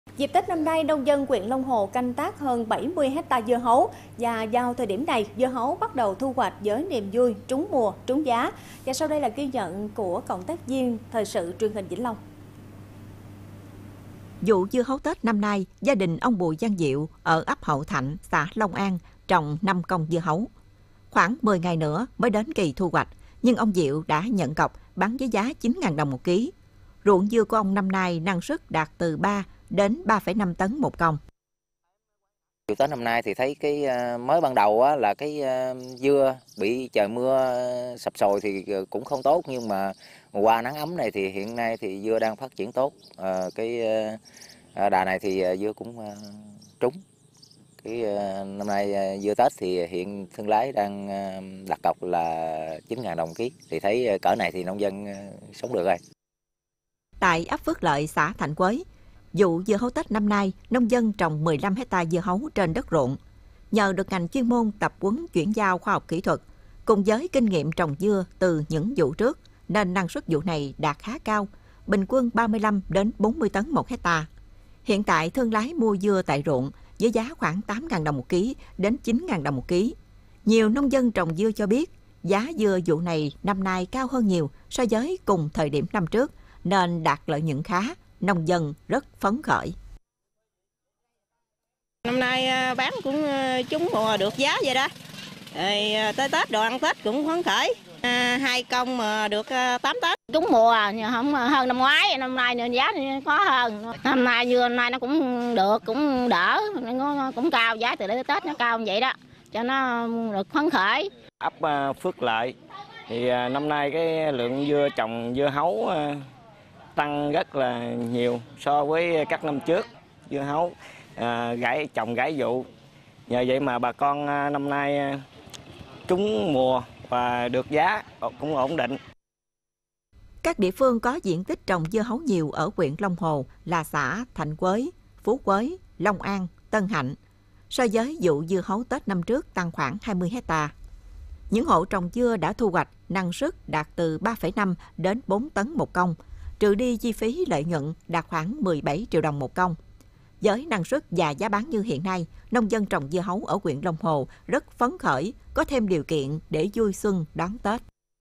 Niềm vui mùa dưa hấu Tết
Dịp Tết năm nay, nông dân huyện Long Hồ canh tác hơn 70 ha dưa hấu, thời điểm này, dưa hấu bắt đầu thu hoạch với niềm vui trúng mùa, trúng giá.

⬇️⬇️⬇️ Tải THVL Audio để nghe đầy đủ